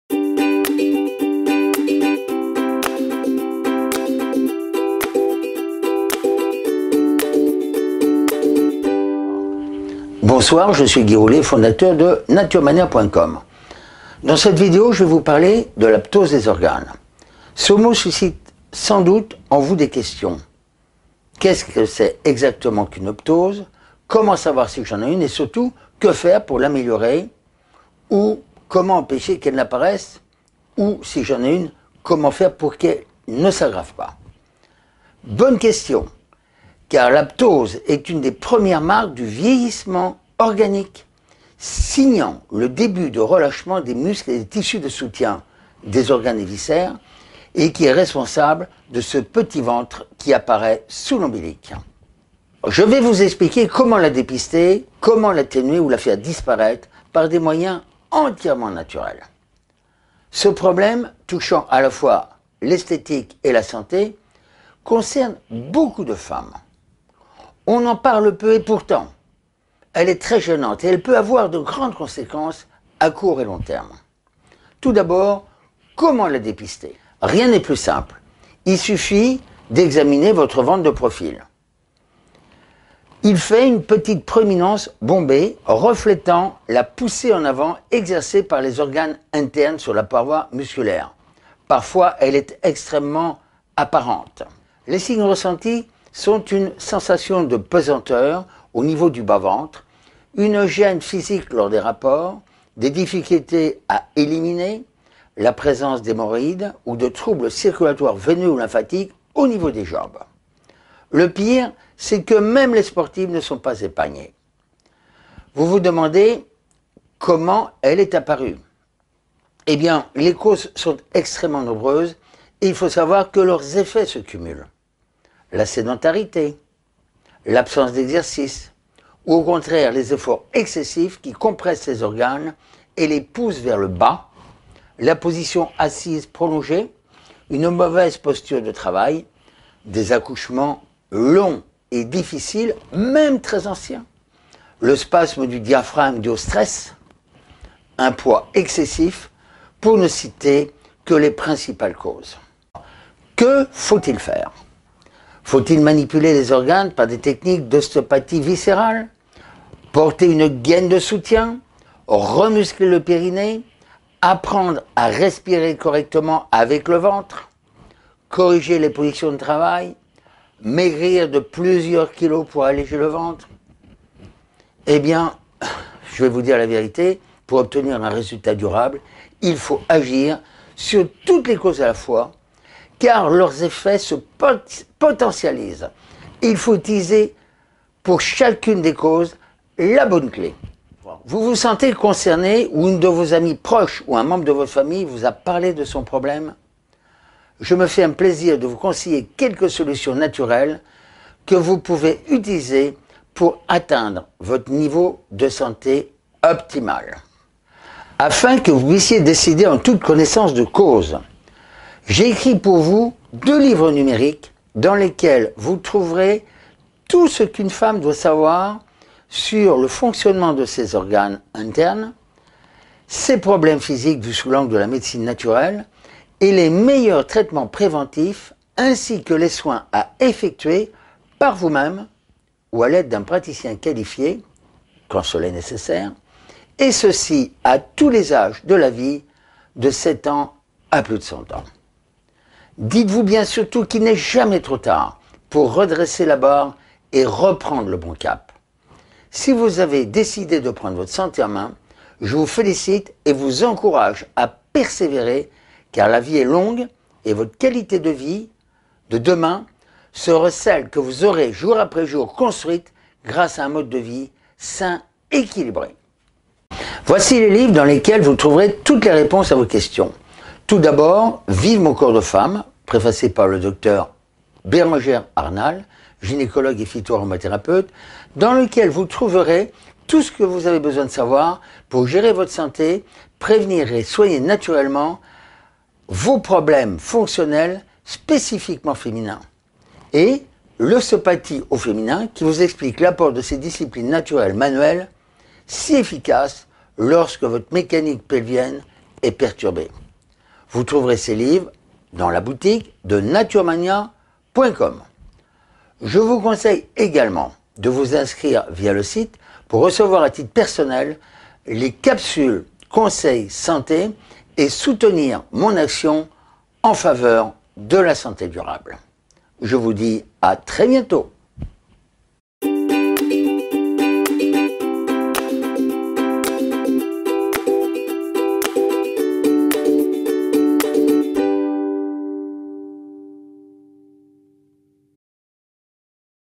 Vidéo Comment soigner naturellement la descente d'organe ou ptôse par Guy Roulier
La ptôse ou descente des organes interne, utérus, vessie, intestins (entéroptose),… concerne un nombre croissant de femmes de tous âges, provoquant des troubles divers plus ou moins gênants. Comment la dépister, se prendre en charge et la traiter naturellement tant qu'il en est encore temps. Cette vidéo vous donne l'essentiel de ce que vous devez savoir pour agir en connaissance de cause et protéger votre capital vital et améliorer considérablement votre aspect physique. A visionner et à faire connaître.